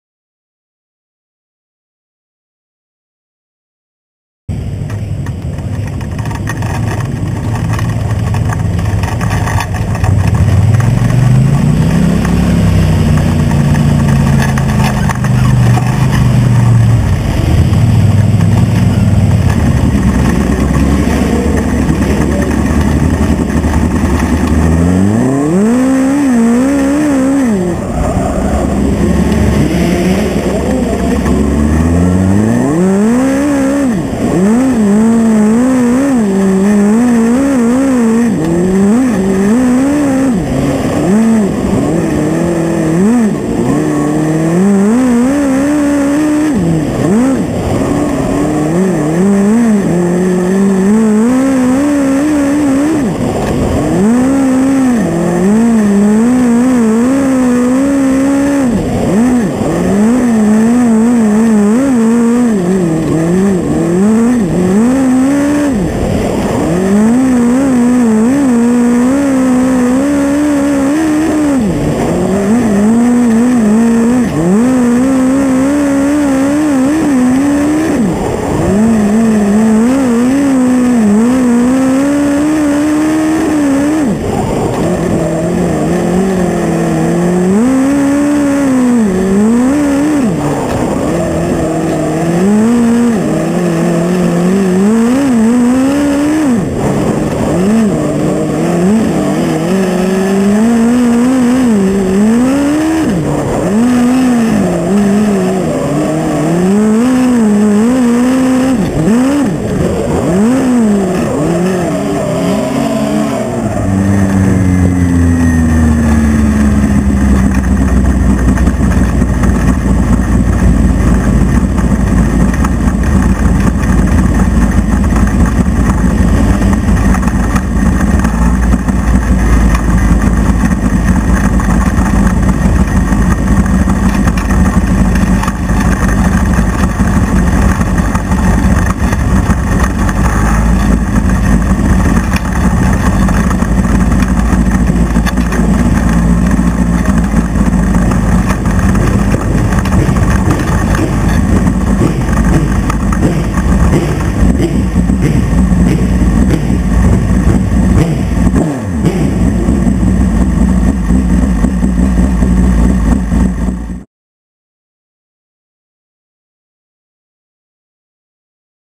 Tyler Thomas 2013 Chili Bowl Practice
Tyler Thomas in the 91T on Practice Day Monday 1-7-13 at the Chili Bowl Midget Nationals in Tulsa, OK. For about Tyler visit: